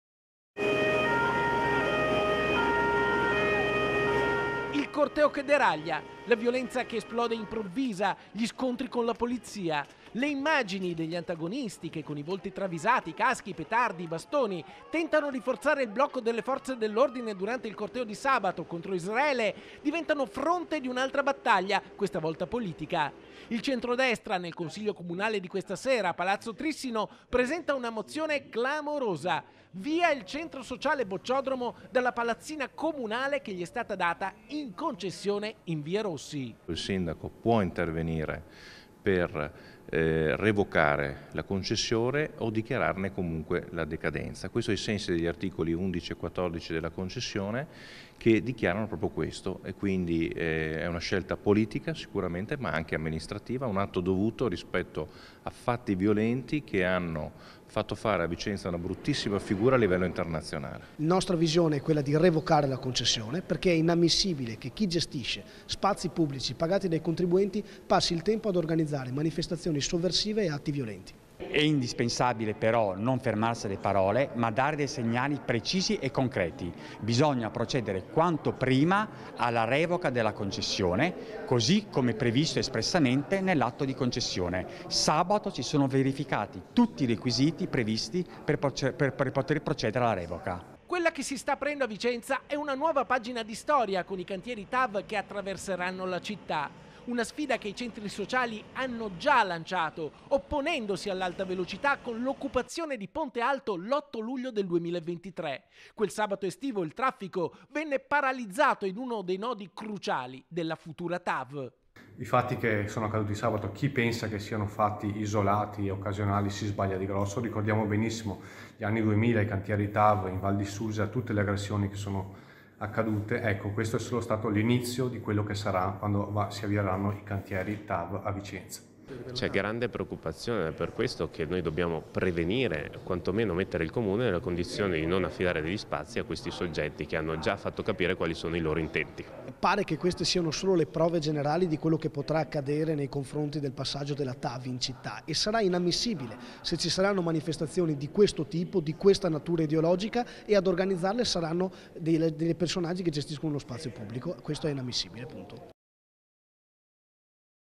VIOLENZA AL CORTEO, IL CENTRODESTRA: «VIA IL CENTRO SOCIALE DAGLI SPAZI COMUNALI» | 23/01/2024
TG Vicenza 23/01/2024 - VICENZA - Dopo gli scontri di sabato a Vicenza, durante il corteo contro Israele, si è aperto un vero e proprio caso politico. Oggi il Centrodestra porta in Consiglio Comunale una mozione in cui chiede la revoca degli spazi pubblici al centro sociale Bocciodromo. - Intervistati FRANCESCO RUCCO (Lista "Rucco Sindaco"), JACOPO MALTAURO (Lega - Capogruppo Consiglio Comunale), VALERIO SORRENTINO (Lista "Rucco Sindaco"), NICOLO' NACLERIO (Fratelli d'Italia - Capogruppo Consiglio Comunale), SILVIO GIOVINE (Fratelli d'Italia - Parlamentare) (Servizio di Carlo Alberto Inghilleri) - Segui Rete Veneta anche sul digitale terrestre! Visita il sito